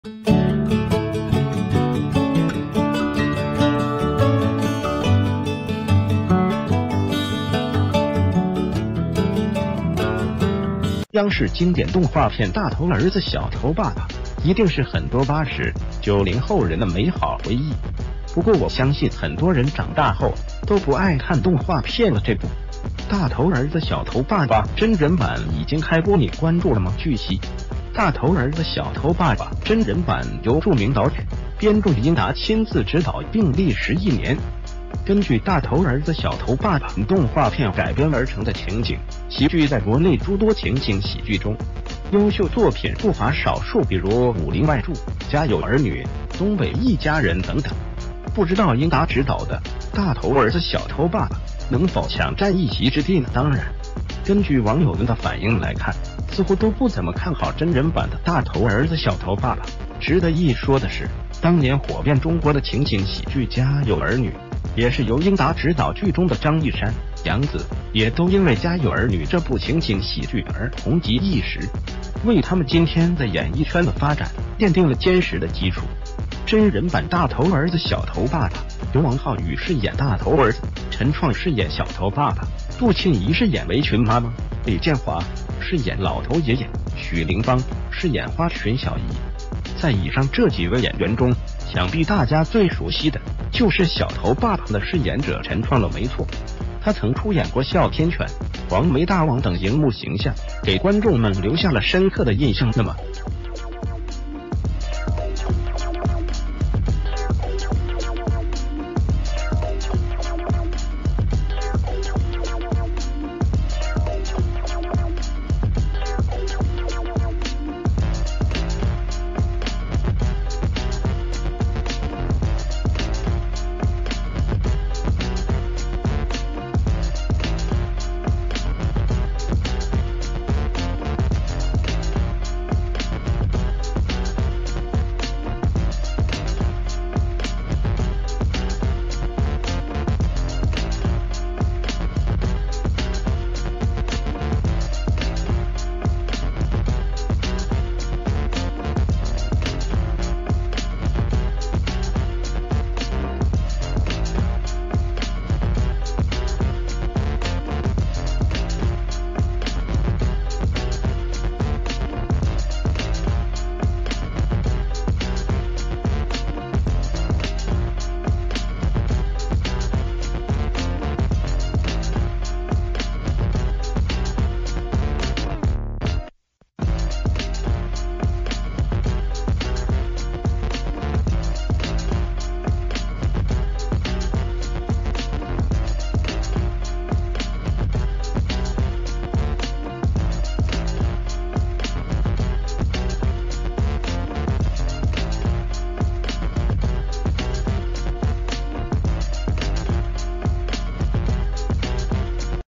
《大頭兒子小頭爸爸》真人版開播，小頭爸爸的飾演者是全劇亮點
谢谢！

    央視經典動畫片《大頭兒子小頭爸爸》，一定是很多80、90后人的美好回憶。不過，我相信很多人長大后都不愛看動畫片了。這不，《大頭兒子小頭爸爸》真人版已經開播，你關注了嗎？
    據悉，《大頭兒子小頭爸爸》真人版由著名導演、編著英達親自執導，并歷時一年，根據《大頭兒子小頭爸爸》動畫片改編而成的情景喜劇。在國內諸多情景喜劇中，優秀作品不乏少數，比如《武林外住》、《家有兒女》、《東北一家人》等等，不知道英達執導的《大頭兒子小頭爸爸》能否搶占一席之地呢？當然，根據網友們的反應來看，似乎都不怎么看好真人版的《大頭兒子小頭爸爸》。
    值得一說的是，當年火遍中國的情景喜劇《家有兒女》，也是由英達執導。劇中的張一山、楊紫也都因為《家有兒女》這部情景喜劇而紅極一時，為他們今天在演藝圈的發展奠定了堅實的基礎。
    真人版《大頭兒子小頭爸爸》由王浩宇飾演大頭兒子、陳創飾演小頭爸爸、杜沁怡飾演圍裙媽媽、李建華飾演老頭爺爺、許齡方飾演花裙小姨。
    在以上這幾位演員中，想必大家最熟悉的就是小頭爸爸的飾演者陳創了，沒錯，他曾出演過哮天犬、黃眉大王等熒幕形象，給觀眾們留下了深刻的印象。
    那么，你對真人版《大頭兒子》小頭爸爸有什么看法呢？請在評論區留言，感謝閱覽。